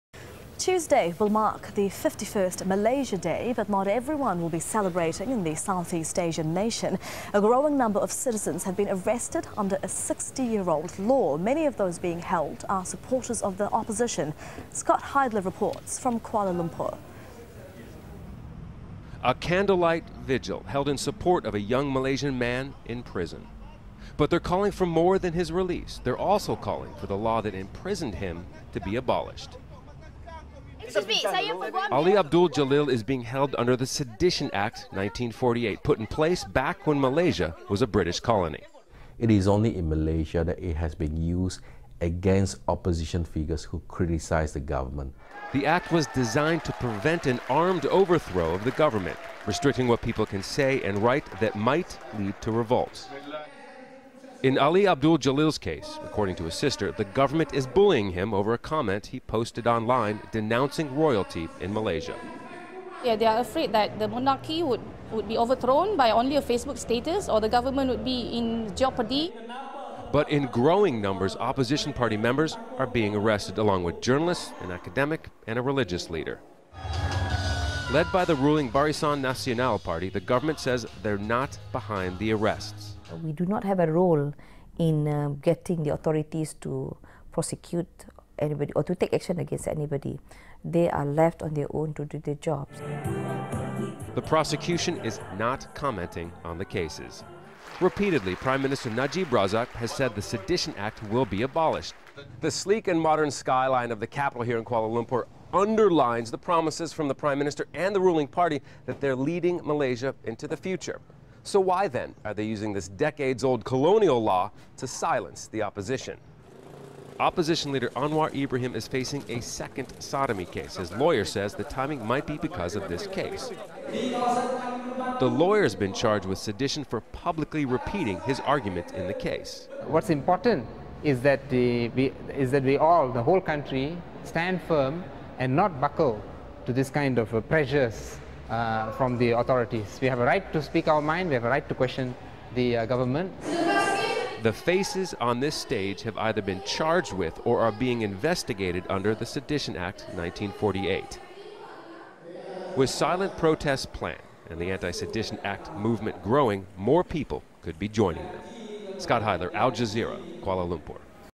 Malaysian government cracks down on dissent
As more are arrested under Malaysia's Sedition Act 1948, established when it was still a British colony, more people are joining the movement to have the laws banned.  Report on Al Jazeera English.